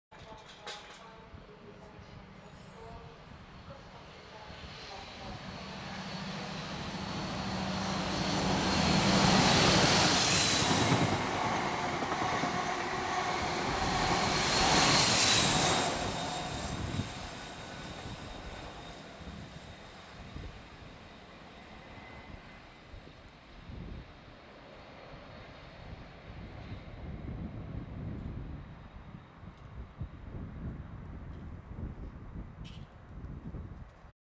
43065 and 43084
Grand Central's 43065 and 43084 scram through Peterborough